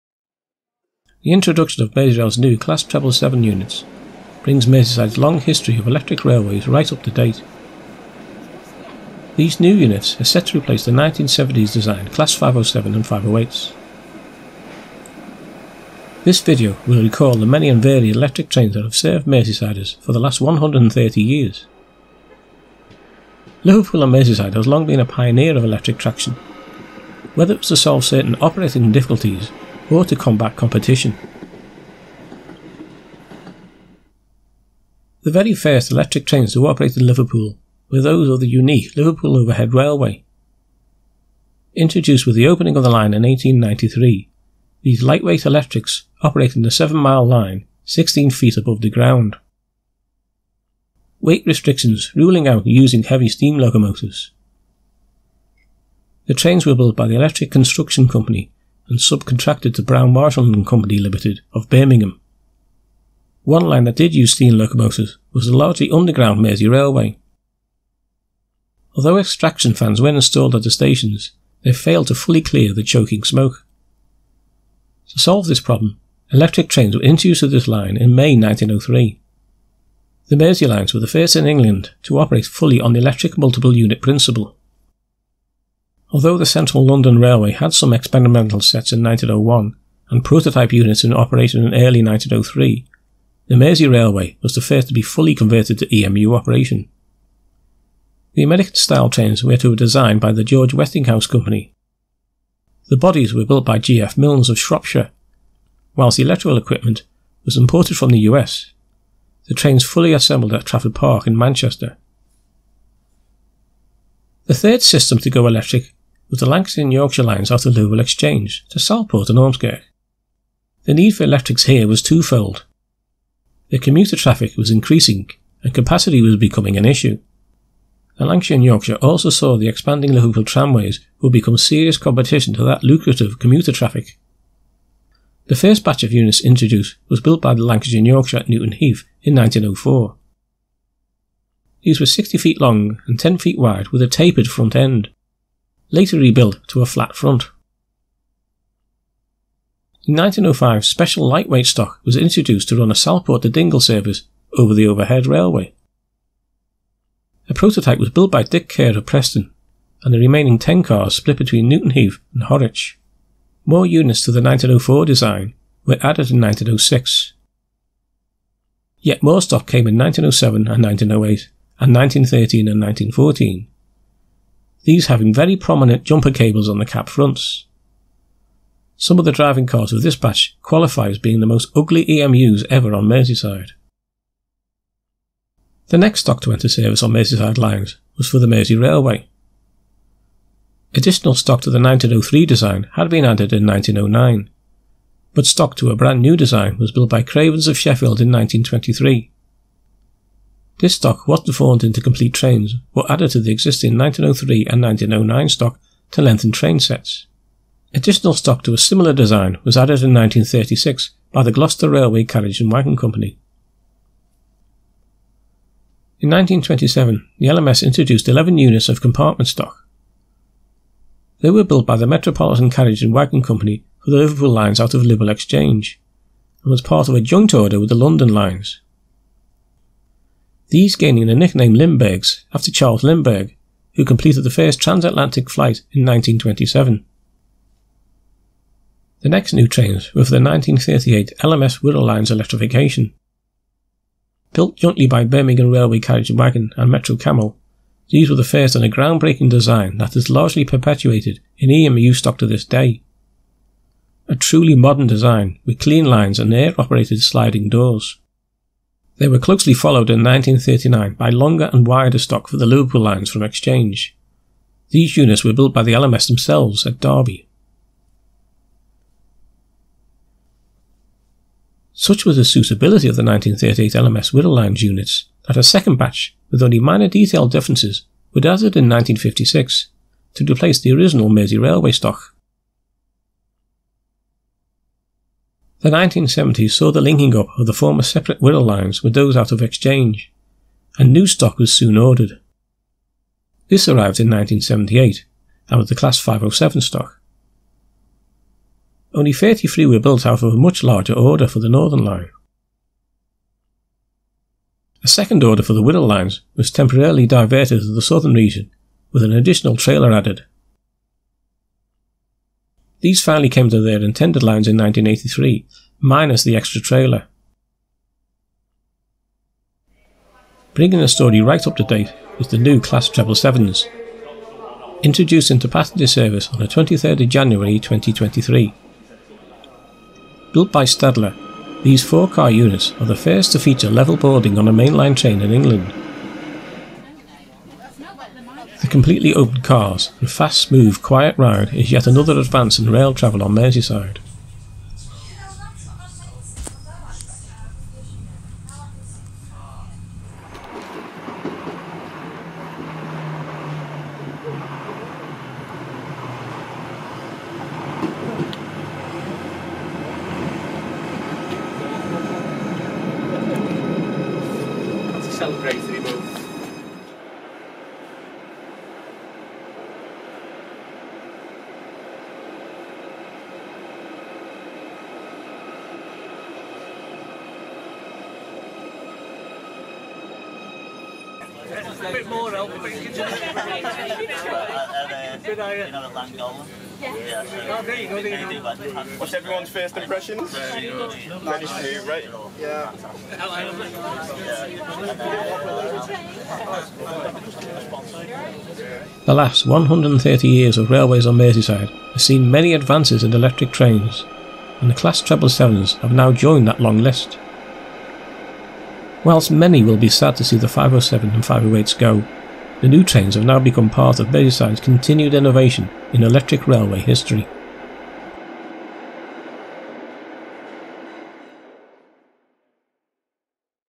Merseyrail Class 777s The Next Generation Of Electrics. Merseyrail History
Merseyrails New Class 777 Electric units join a long line of Merseyside innovation in electric railway history. This video charts the 130 years of Merseyside electric trains history from the first Overhead railway trains right through to the latest class 777s